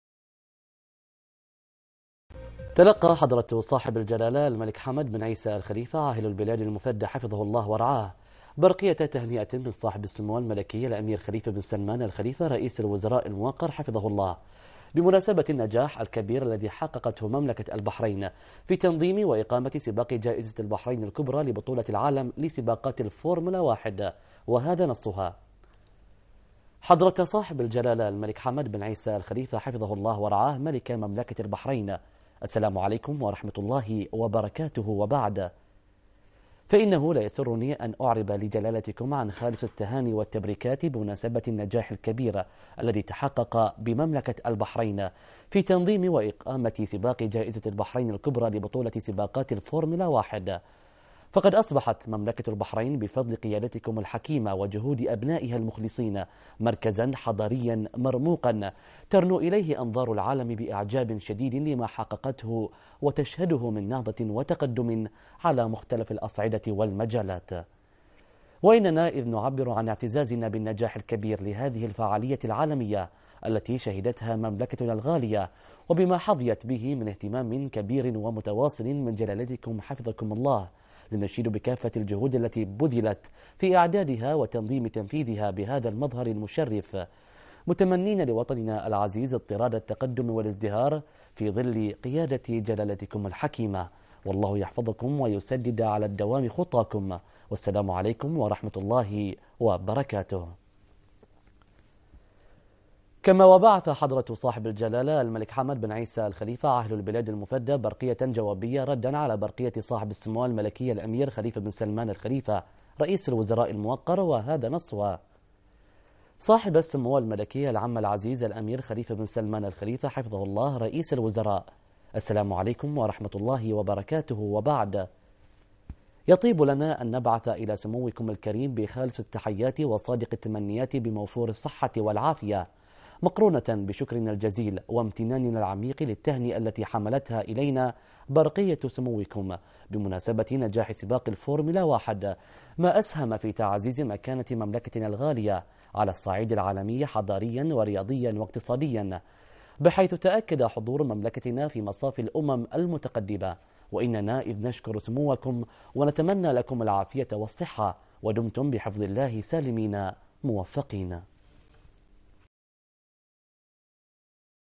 البحرين: عاهل البلاد المفدى يتلقى برقية تهنئة من سمو رئيس الوزراء بمناسبة نجاح سباق الفورمولا 1
المنامة في 3 ابريل / بنا / تلقى حضرة صاحب الجلالة الملك حمد بن عيسى آل خليفة عاهل البلاد المفدى حفظه الله ورعاه برقية تهنئة من صاحب السمو الملكي الأمير خليفة بن سلمان آل خليفة رئيس الوزراء الموقر حفظه الله ورعاه، بمناسبة النجاح الكبير الذي حققته مملكة البحرين في تنظيم وإقامة سباق جائزة البحرين الكبرى لبطولة العالم لسباقات فورمولا (1) في فاتحة سباقات هذا العام، هذا نصها: